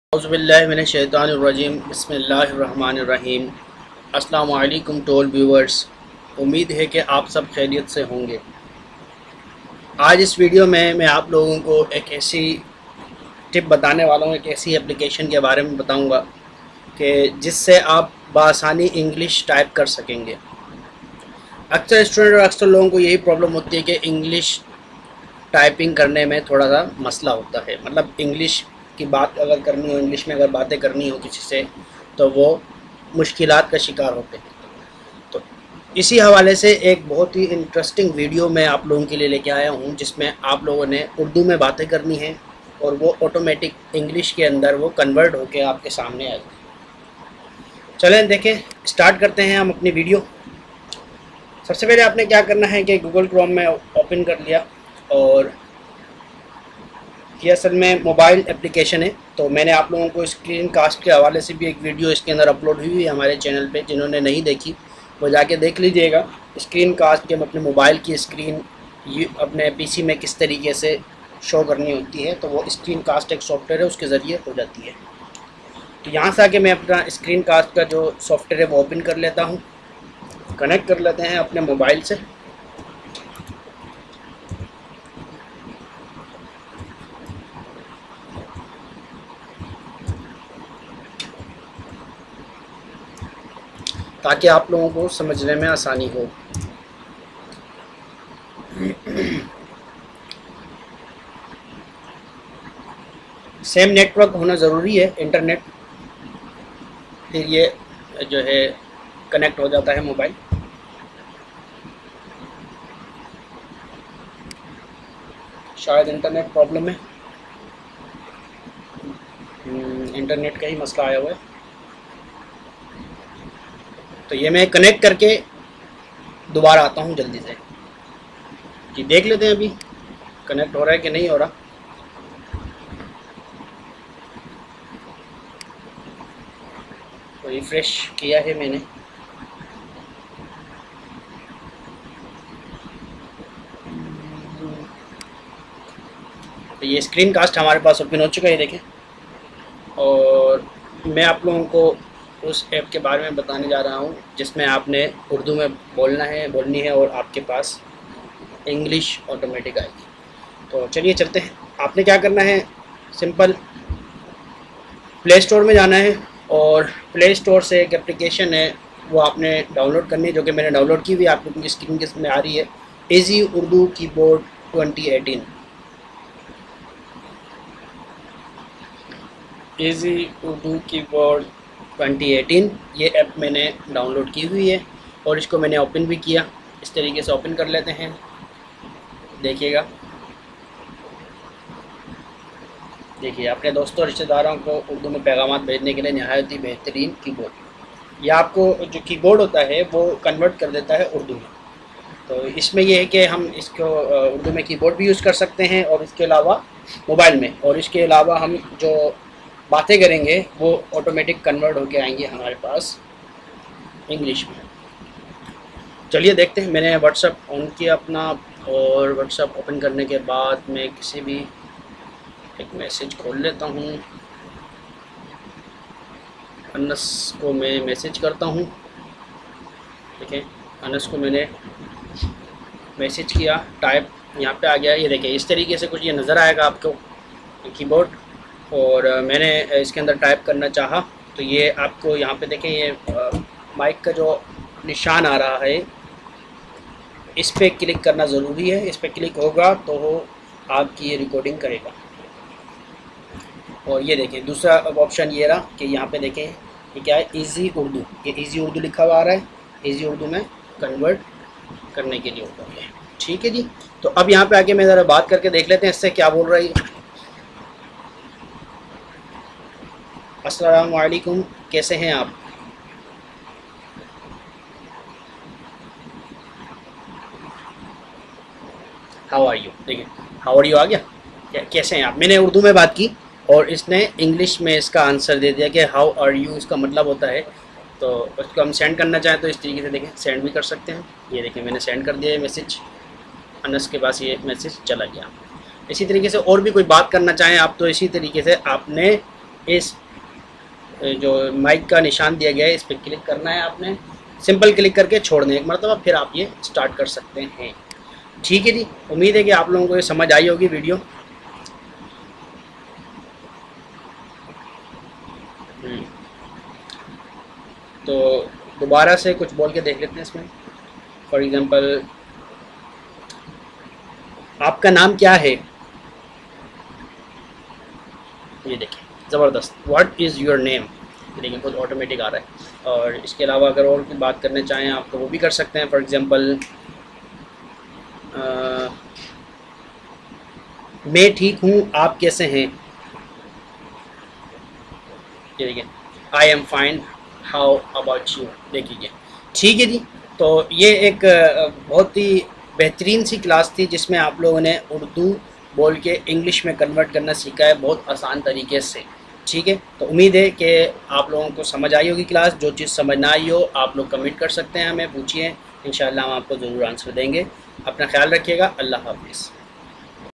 "Urdu to English Translation Made Easy | Learn How to Translate Urdu to English"
In this tutorial, you will learn how to translate Urdu to English using different methods and tools. We will explore online translation services, software applications, and even manual translation techniques to help you understand Urdu text and convert it into English. Whether you're a student, a professional, or simply interested in Urdu language and culture, this video will provide you with valuable insights and resources for translating Urdu to English.